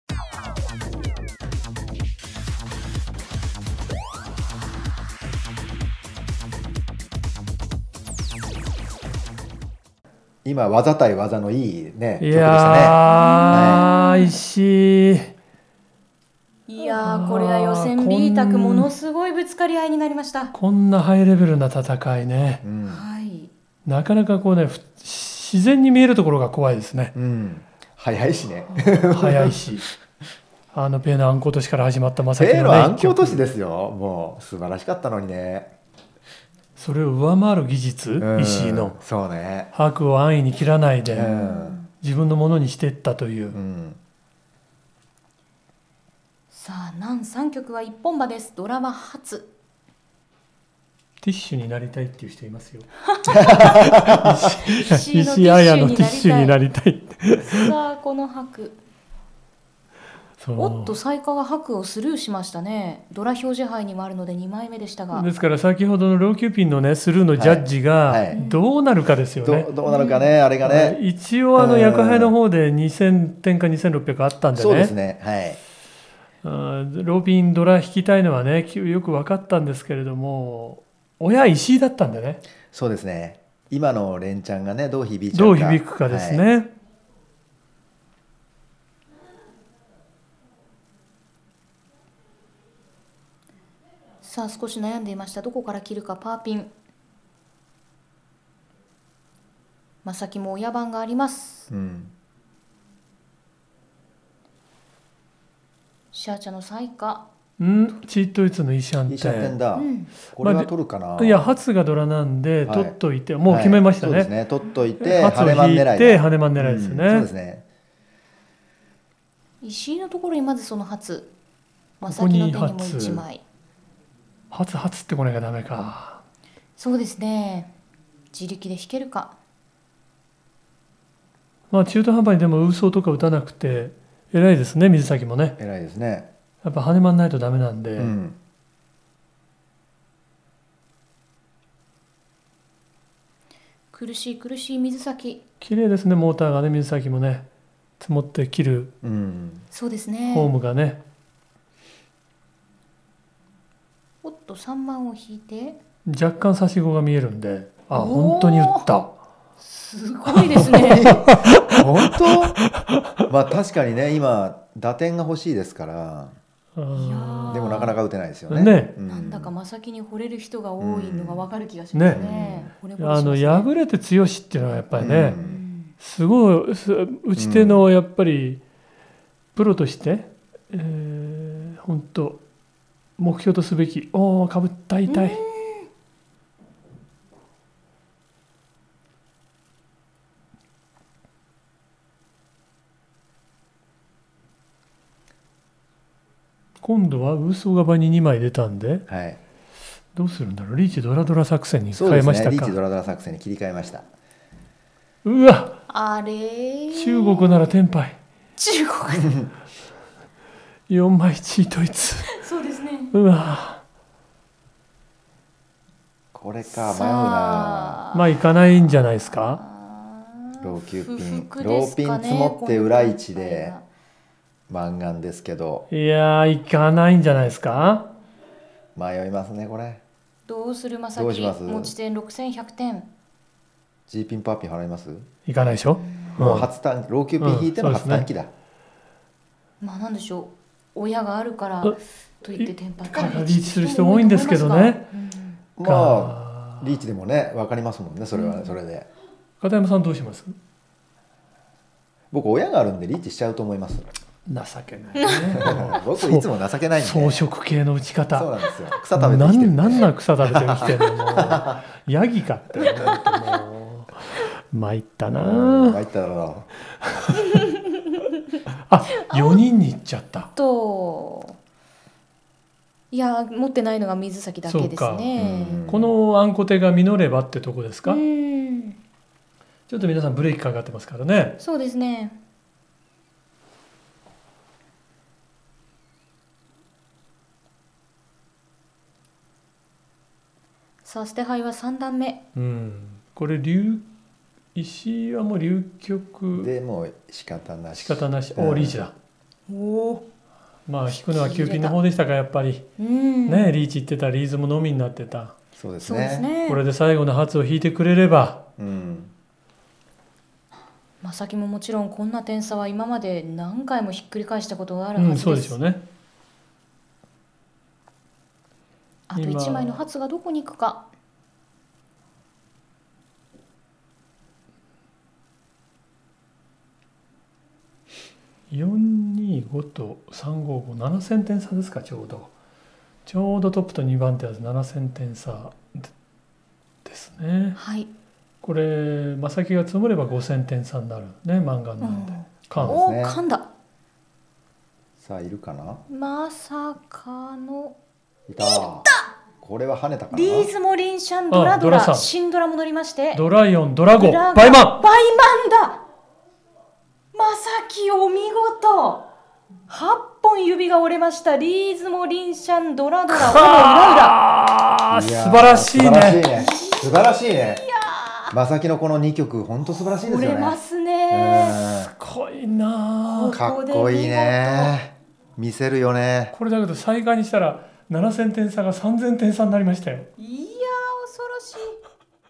【麻雀】眞崎雪菜：執念のリンシャンカイホウ！
四神降臨2014女流王座決定戦にて、日本プロ麻雀協会の眞崎雪菜プロが会場を嶺上開花で沸かせた！
こんなに熱いリンシャン牌はそうそうない！？

この対局番組が気になったアナタ！